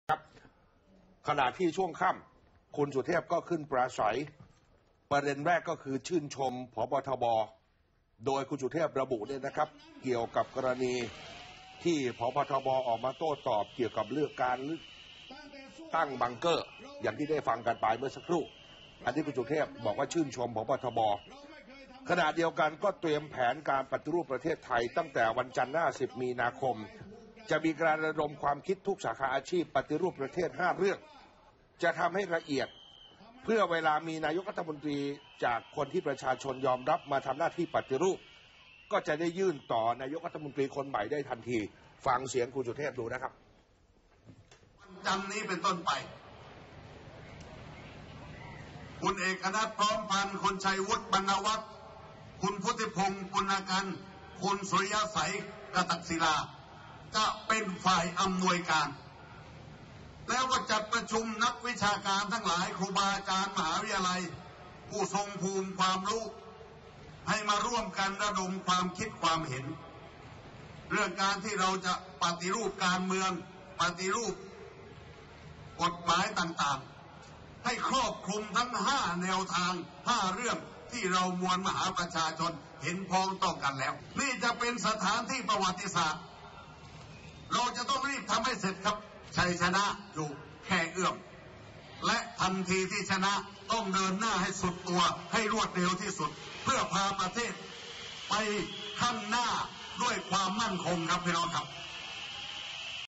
สุเทพ เมินเฉลิมท้าเจรจา ชวนคนปฏิรูป ปท เริ่ม10มี ค
▶ Twitter (โฟโล้ ทวิตเตอร์) ▶
▶ Website (เข้าชมเว็บ) ▶
นายสุเทพ เทือกสุบรรณ ปฏิเสธไม่ขอรับนัดพูดคุยกับร.ต.อ.เฉลิม อยู่บำรุง พร้อมประกาศเดินหน้าปฏิรูปประเทศ ตั้งแต่วันจันทร์ที่ 10 มีนาคมเป็นต้นไป -